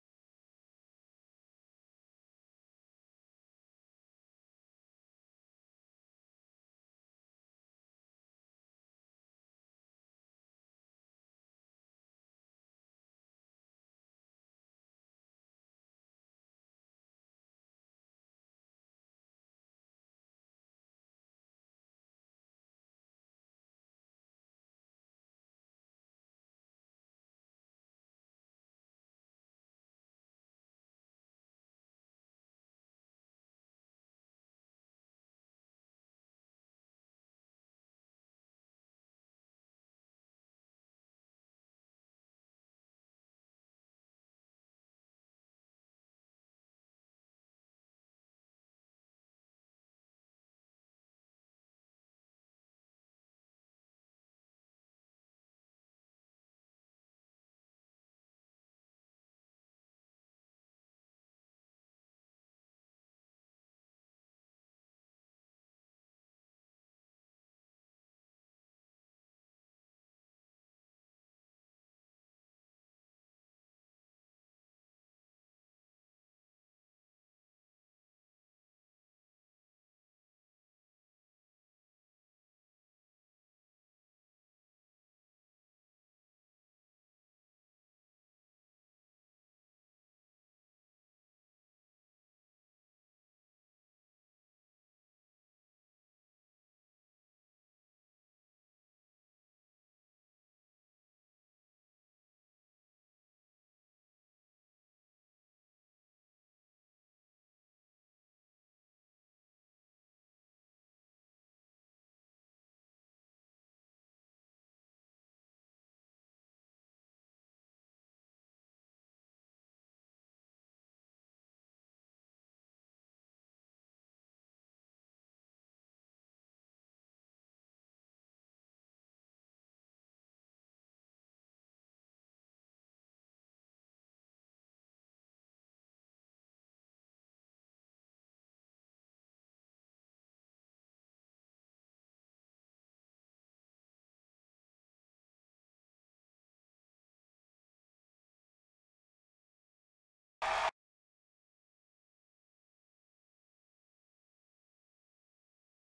Pontiac G8 GT Autocross Griesbach Edmonton
2 passes of me going around the course.